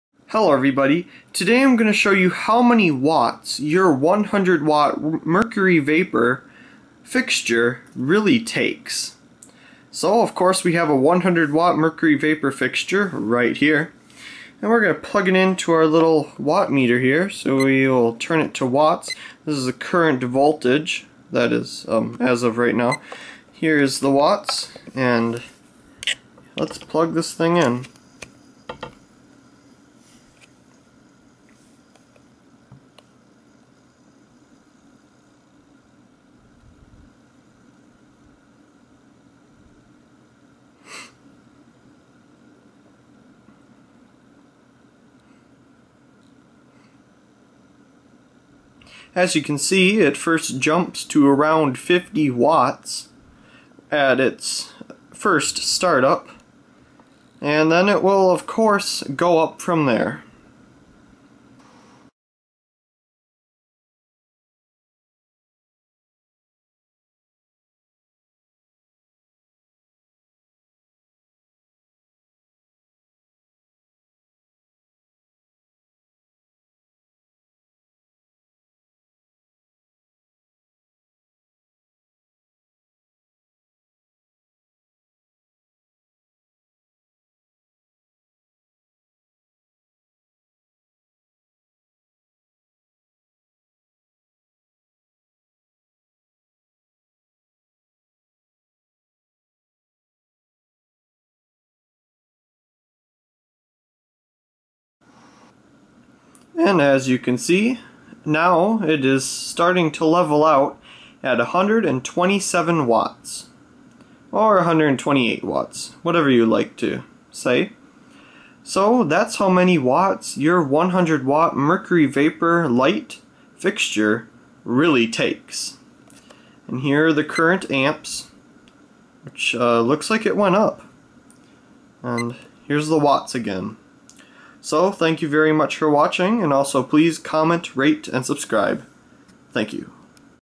Real Watts - 100watt Mercury Vapor (MV) fixture
Here are the Real Watts of a 100watt Mercury Vapor (MV) fixture.